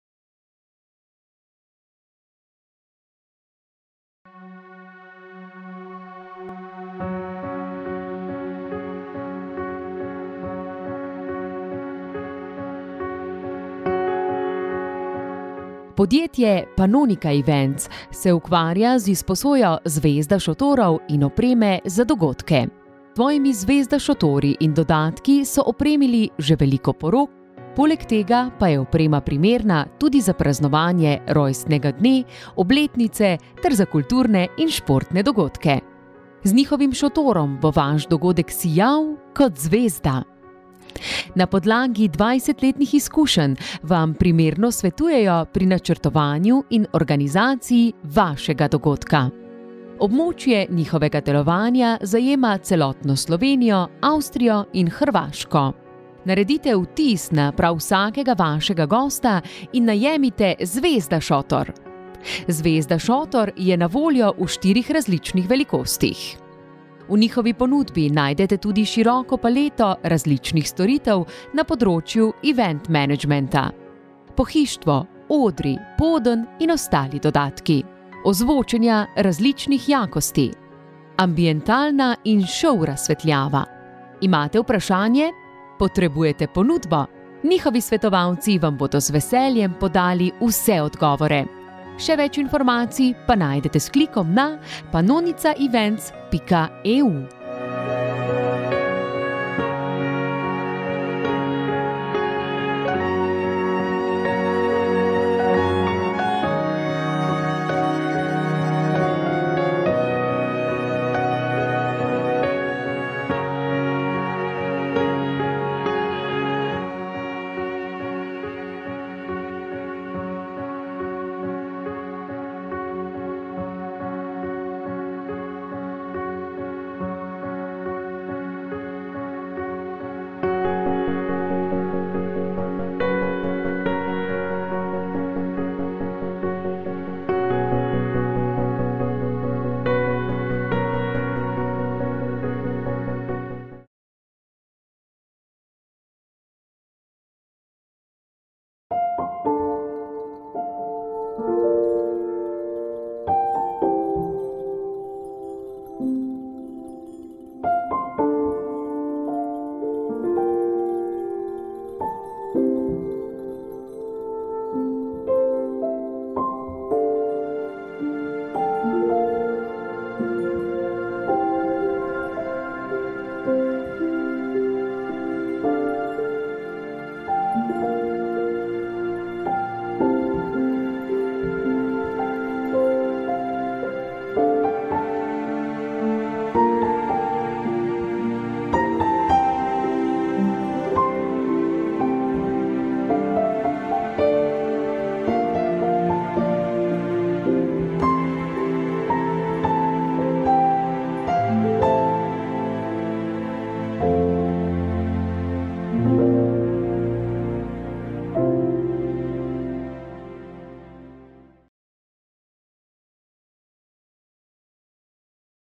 🌸SLOVENSKI POROČNI SEJEM 2021 🌸 Panonica Events
🌸 G O O D   V I B E S    O N L Y 🌸
🌺Hvaležni, veseli in ponosni smo, da nam je uspelo izpeljati tako edinstven poročni sejem!🌺
💓 Uspel nam je krasen vikend! Pod streho smo spravili že četrti Slovenski poročni sejem. Sicer tokrat spletni, ampak vseeno vrhunski.💓
Kaj pravite? Vam je bilo všeč?
♥️Hvala vsem, ki ste nas spremljali in nam z vašimi pozitivnimi odzivi dali še več zagona ❤️
❤️Hvala vsem poročnim ponudnikom, ki so se odzvali povabilu in imeli zaupanje v našo ekipo. Tudi zaradi vas imamo sedaj odlično vsebino ❤️
MARATONSKA EKIPA
🌸 Voditeljica - Mina Pal - MiniNasveti.si
🌸 Projektni vodja, scenografija - Nina Pavec -SKEDENJ
🌸 Režiser - JANI PAVEC - Wedding Crashers Band
🌸 Asistentka režije, kamera - Mateja Berguš - Studio Boutique
🌸 Kamera - Dejan Berguš - Studio Boutique
🌸 Zvok - Miha Novak - Bikini Detectors
🌸 Realizator - Miran Rupar - NajemiDJa.si
🌸 Osvetlitev, skrbnik spletnih strani - Jože Rupar - Dj Yoco